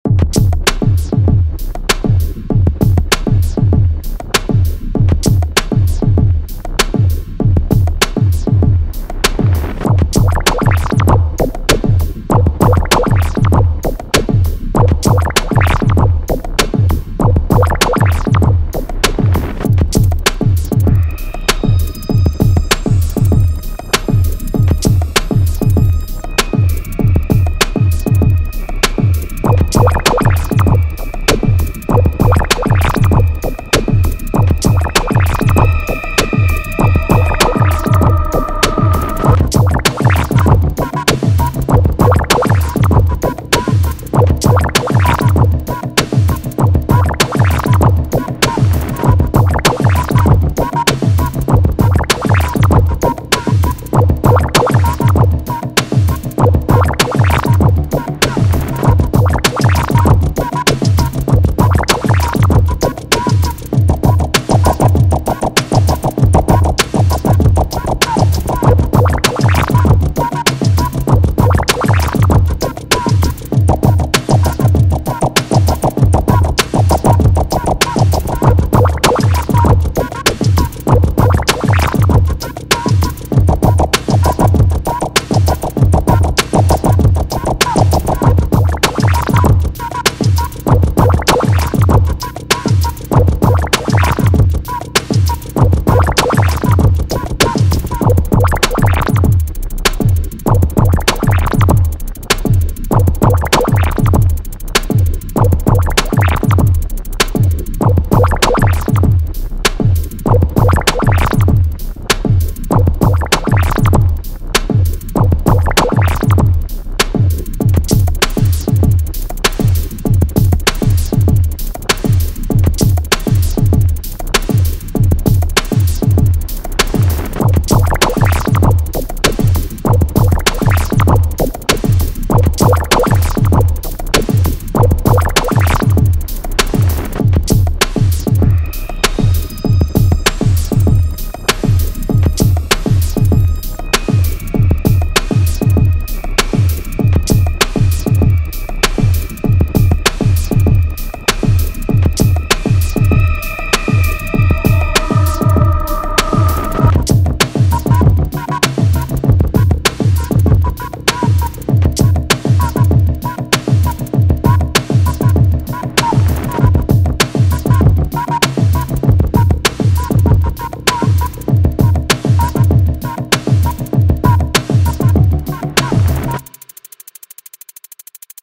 UnBoxing Switch On Chopper CH-B0101
UnBoxing Switch On Chopper CH-B0101, 2 operating speeds, capacity 500 ml, different colors, 260 W from Kaufland